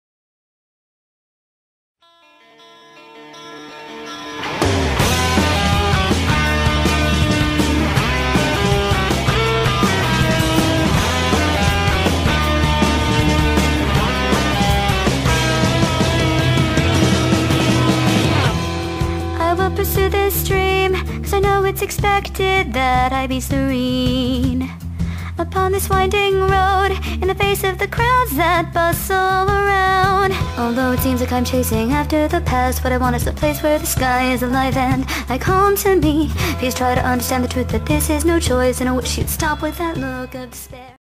something to say about FMA
ok so i made a new account for my fma vids i dont want to use this one for both so im gonna use AnimeElric for anime,im gonna still use this one so dont switch on me